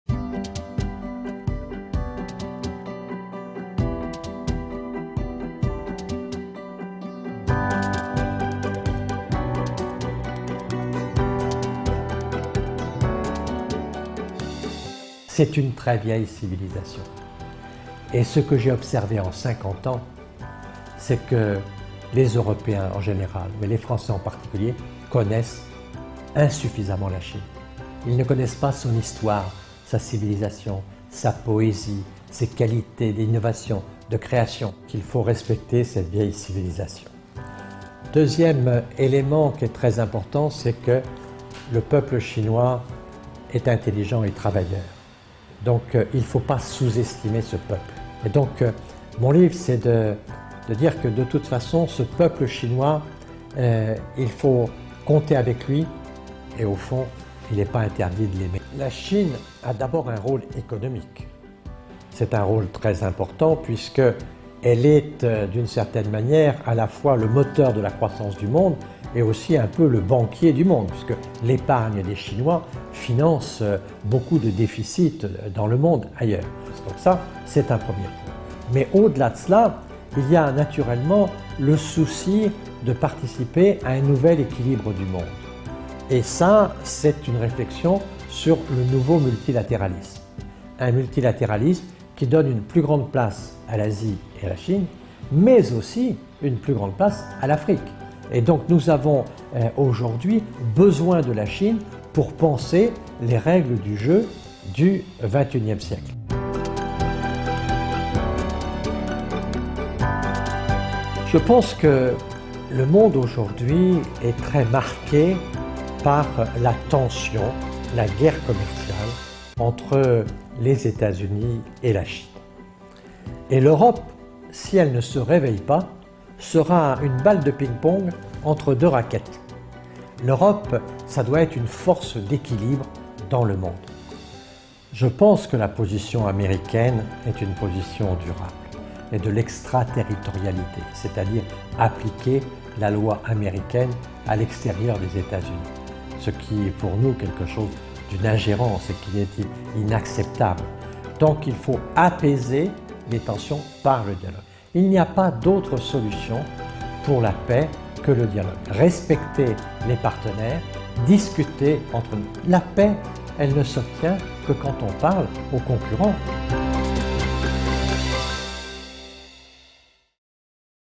Former French PM calls for China's participation in building new international rules
Former French Prime Minister Jean-Pierre Raffarin published a new book about China recently. He says that China contributes to renewal of multilateralism in a world troubled by a strong resurgence of unilateralism.